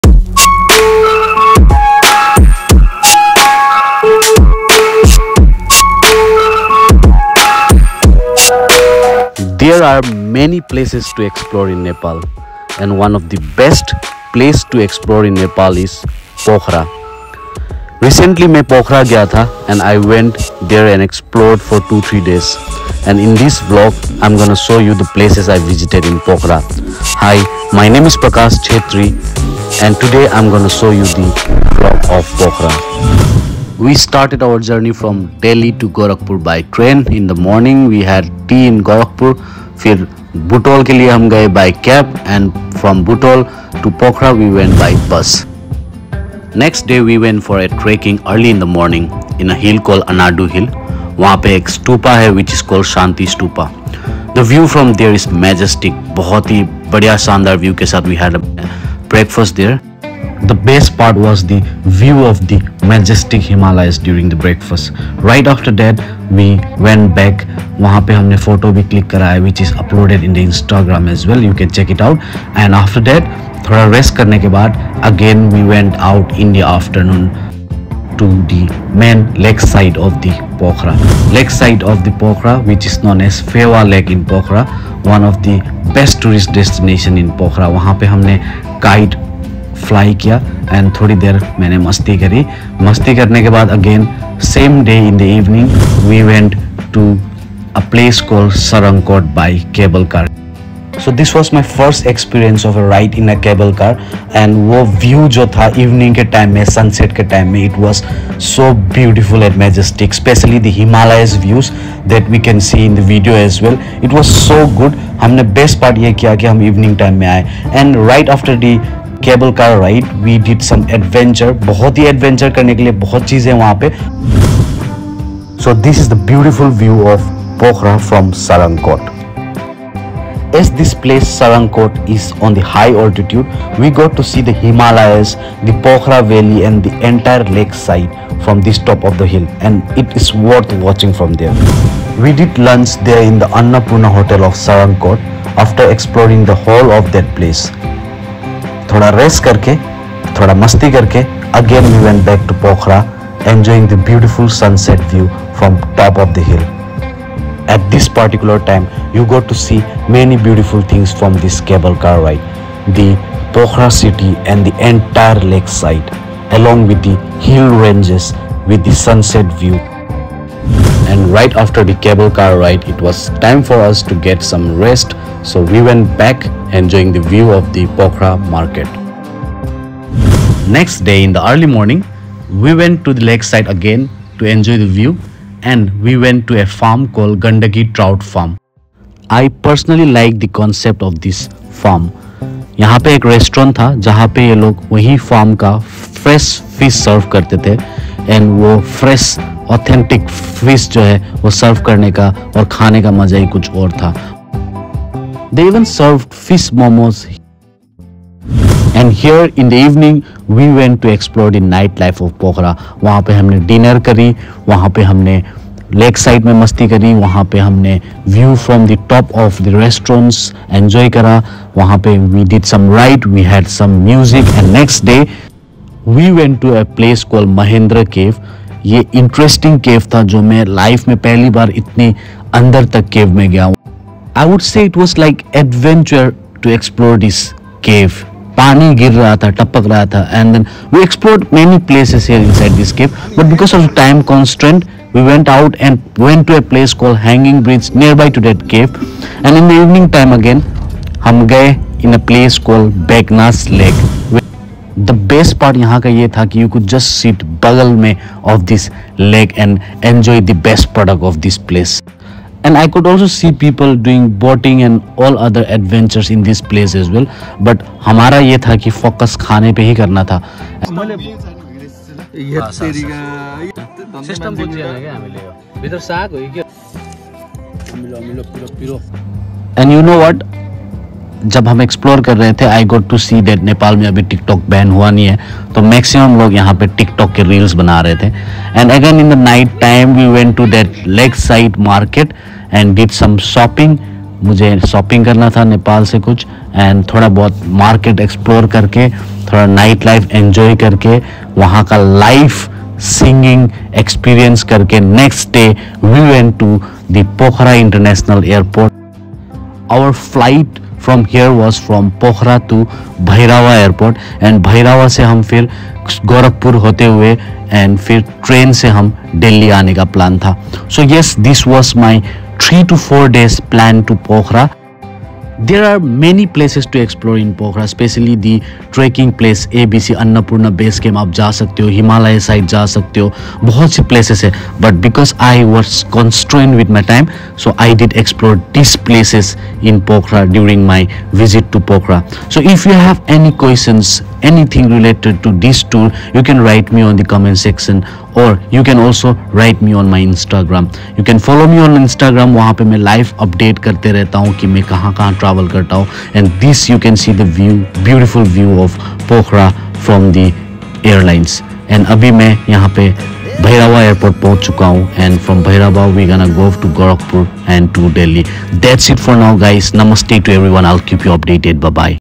Places to visit in Pokhara || India to Nepal || Prakash chhetry
Pokhara is a beautiful city located in Nepal, renowned for its stunning natural scenery, including the picturesque Phewa Lake, the majestic Annapurna mountain range, and the tranquil atmosphere. Travelers often visit Pokhara for outdoor activities like trekking, paragliding, and boating.